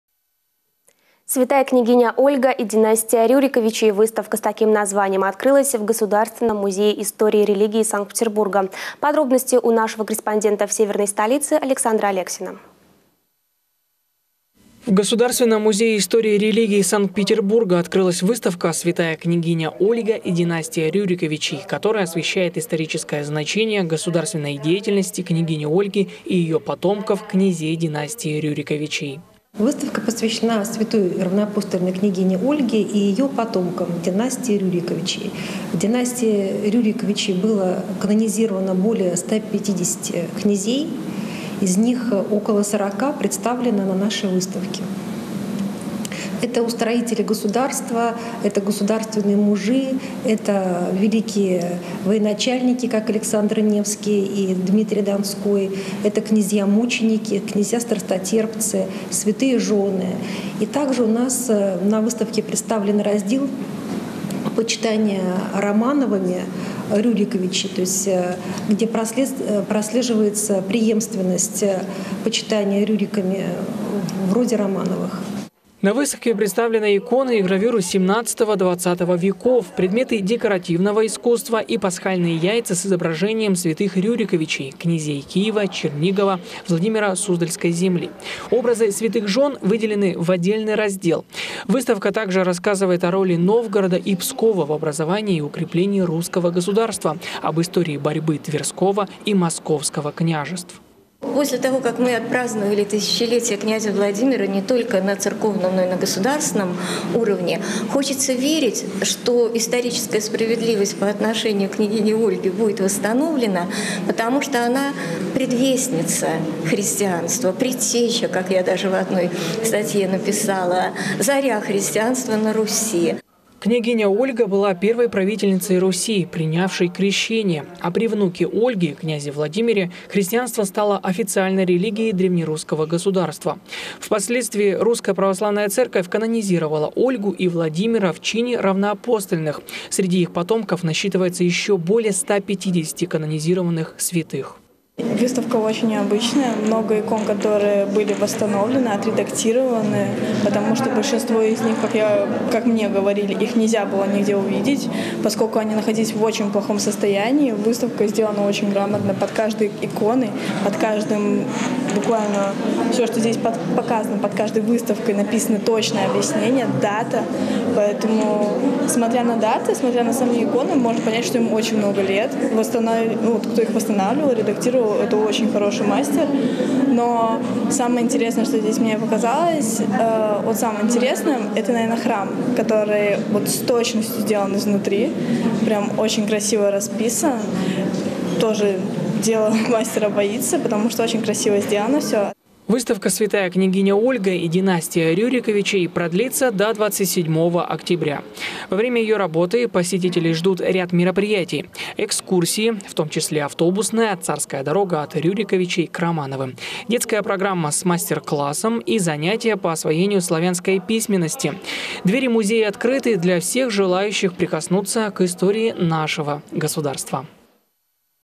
Выставка «Святая княгиня Ольга и династия Рюриковичей» открылась в Санкт-Петербурге
«Святая княгиня Ольга и династия Рюриковичей» выставка с таким названием открылась в государственном музее истории религии Санкт-Петербурга.